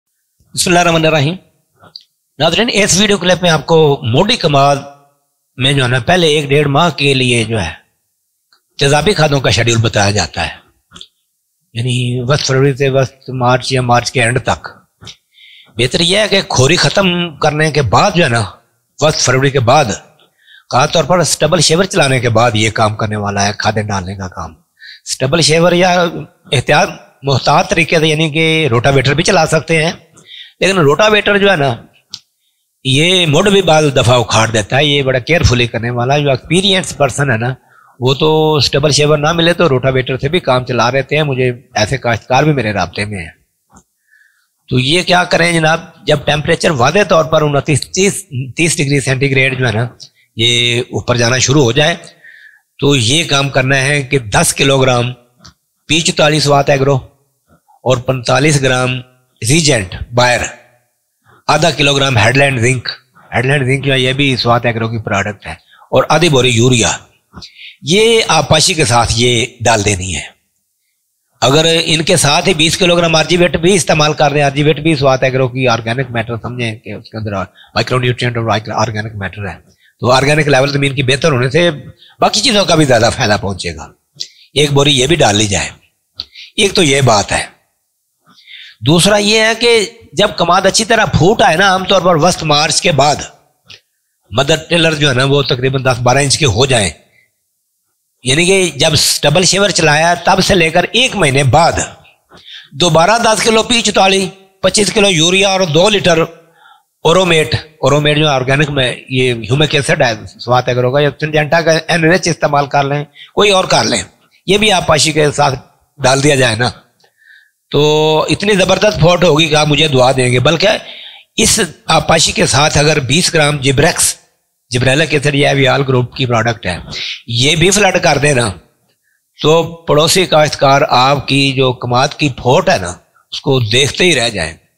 Acidic Fertilizer Schedule for Ratoon crop of Sugarcane || Crop Reformer
Almost 40-50 % sugarcane is kept as ratoon crop in Punjab but it produces 25-30 % less sugarcane than newly planted crop in most cases. In this video, we have guided the farmers to use stubble shaver after harvesting of sugarcane crop. Rotavator may also be used carefully in place of sugarcane stubble shaver in connection with better management of ratoon crop. Stubble shaver does not only increase its ratoonability, but control of insect, disease and weeds is also promoted with this novel implement. Zulfiqar Cheema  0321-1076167, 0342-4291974 is a successful sugarcane grower and he has been using rotavater in place of stubble shaver for better sprouts of ratoon crop. He succeeds to convert his ratoon crop into just like plant crop.
Stubble shaver going to become the essential part of production technology of sugarcane. It converts the ratoon crop of sugarcane into a new healthy crop to make the production higher enough to meet the targets set by the farmers. Use of stubble shaver guarantees the higher yield, growth and development of plants. This technique should be adopted widely to increase the overall yield of ratoon crop of sugarcane to make it more profitable and beneficial for economy of our country.